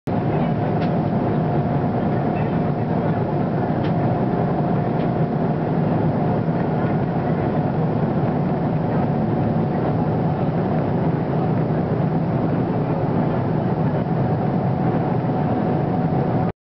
b777 enroute
diameter of 777 turbines more than 4 meters, i guess wings lifted at tips